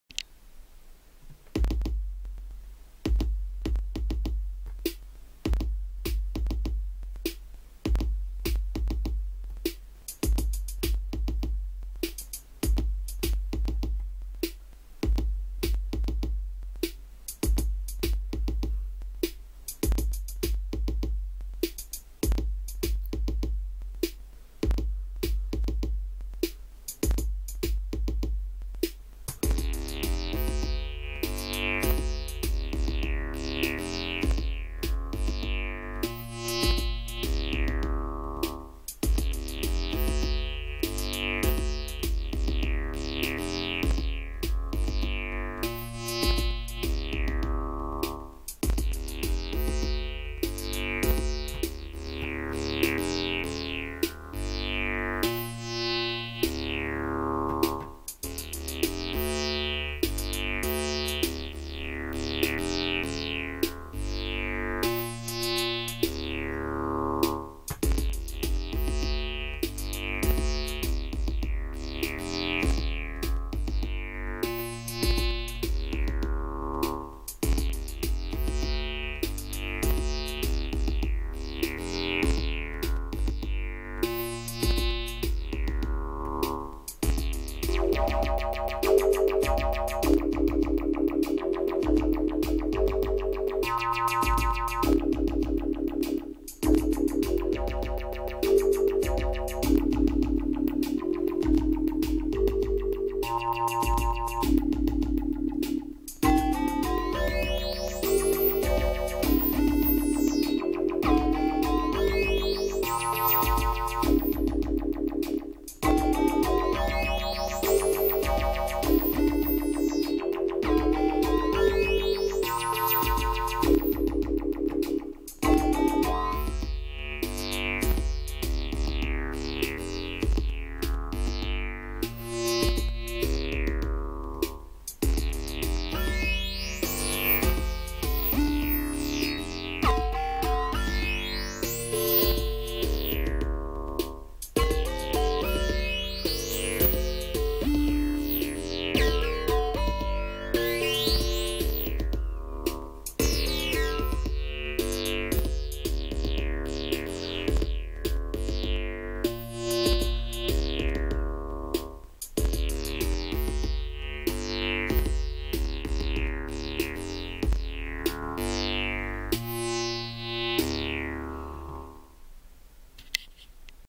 Novation Circuit Beat #1
One of the first beats I created using the Novation Circuit